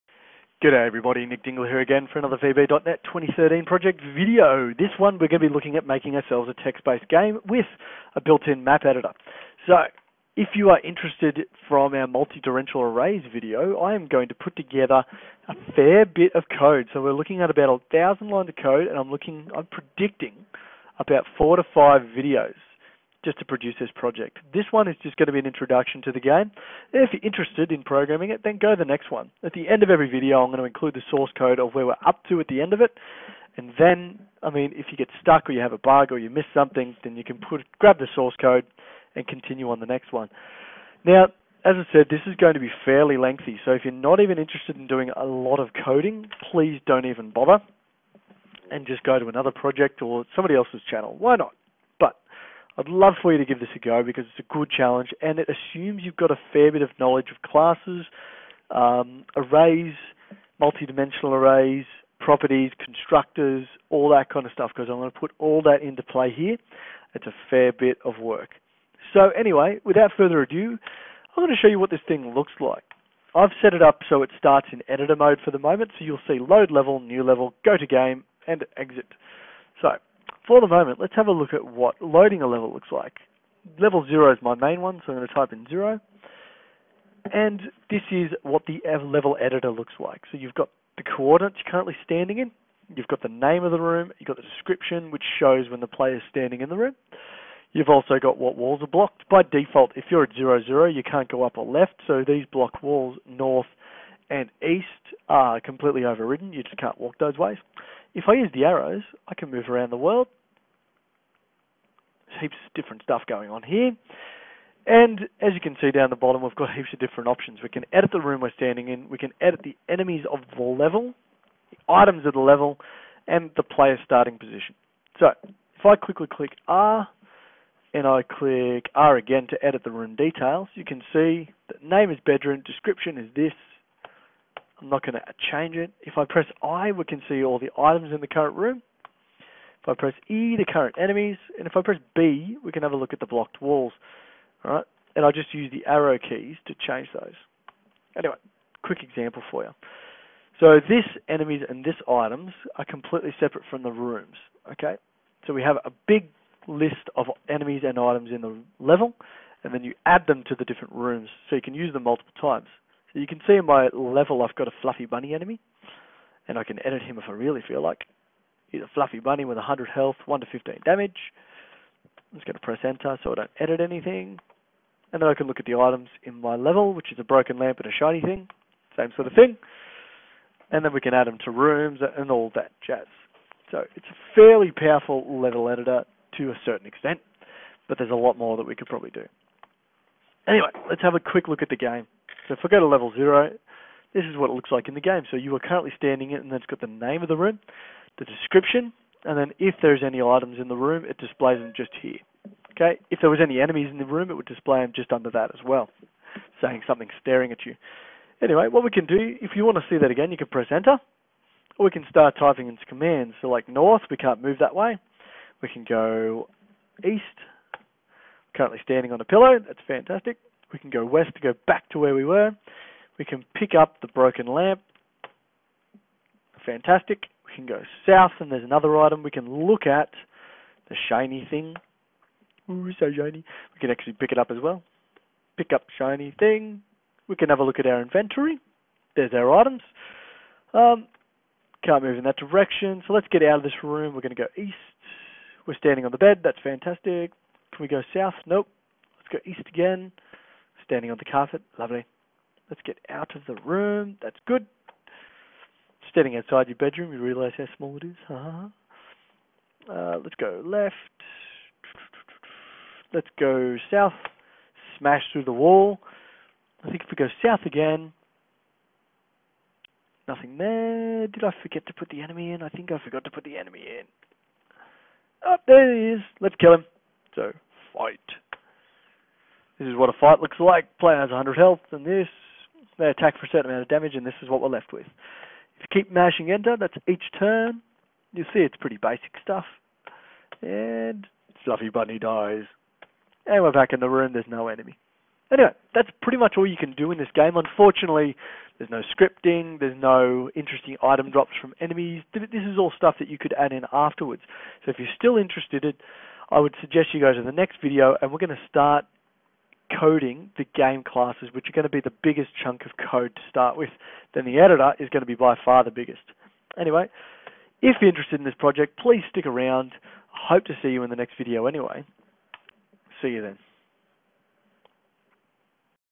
VB.NET 2013: Text Based Game (Part 1/6)
A series dedicated to all those console application nerds out there, how to make yourself a full fledged text based video game. The tutorial takes you through how to begin setting up the background storage, the game's level editor and finally the game.

This video covers:
 * An introduction to the project
 * What you will be making